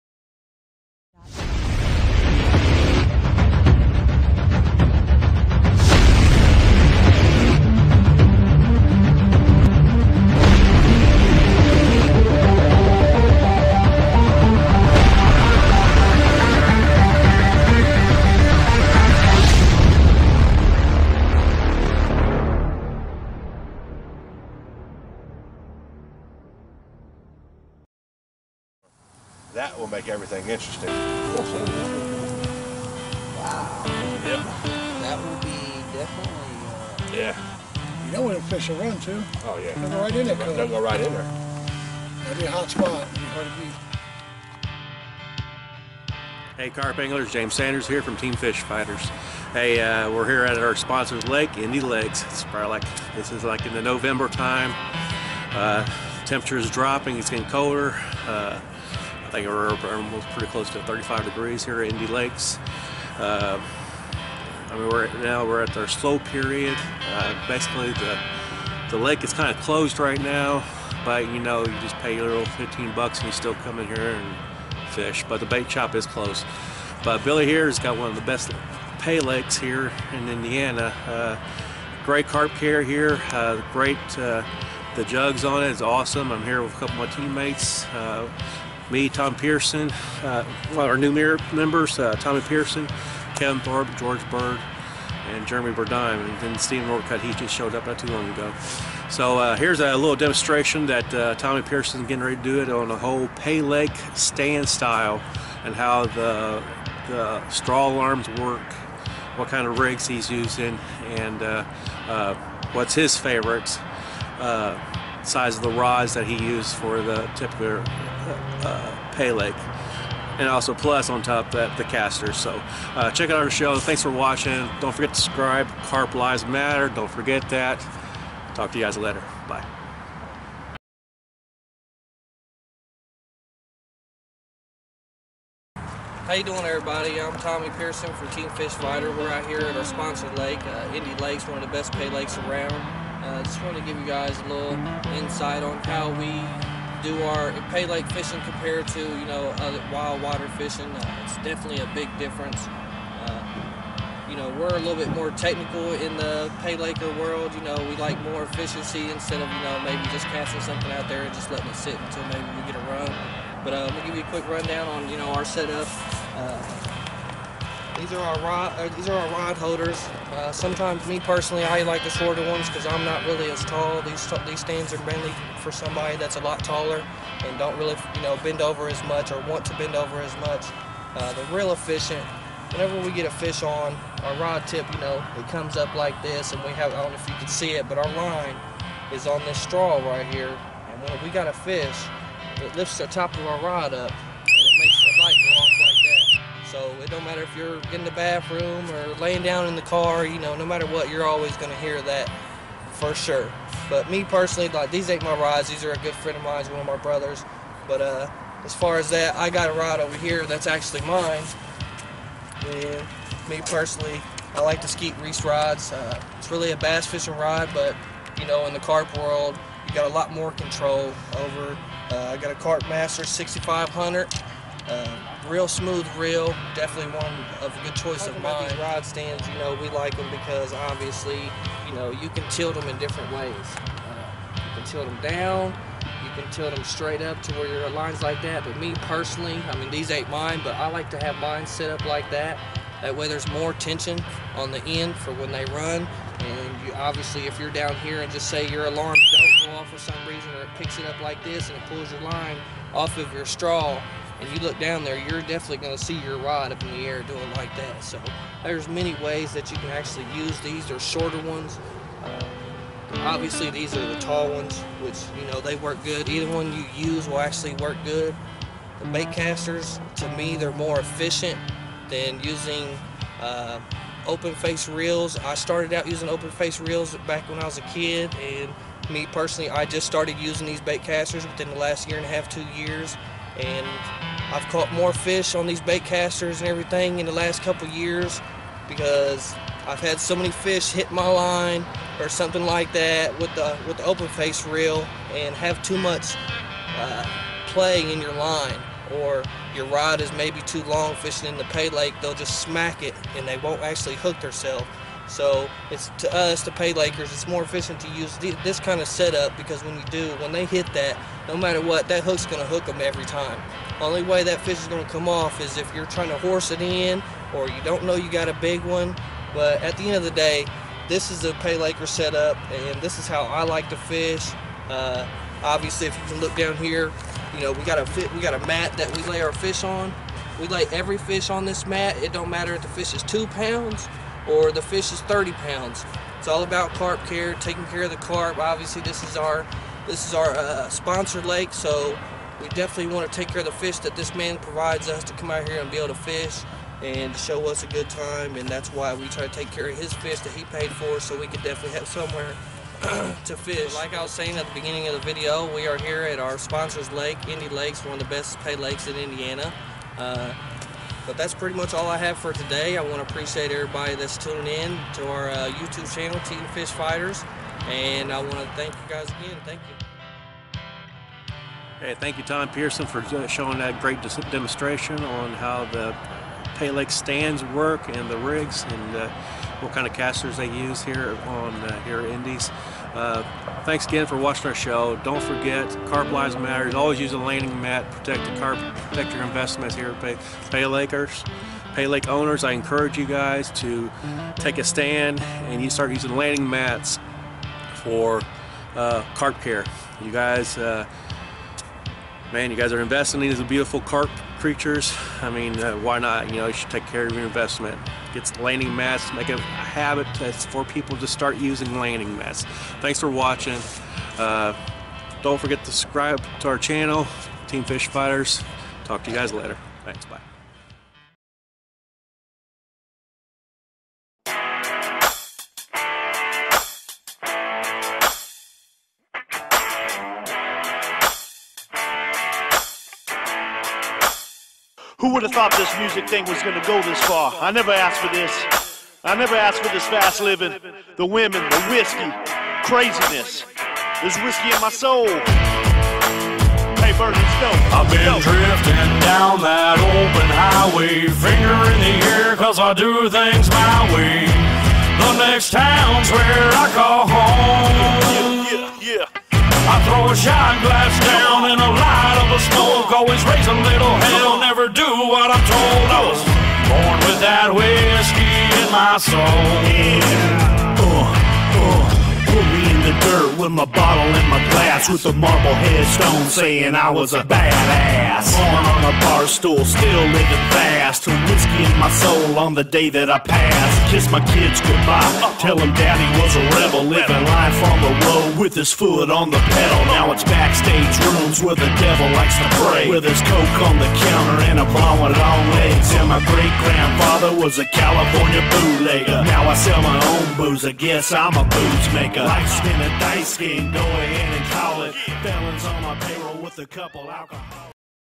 Paylakers equipment setup
Team Fish Fighters
Pro staff Tommy Pearson, showing how the paylakers tilt stand, Casters and straw alarms works.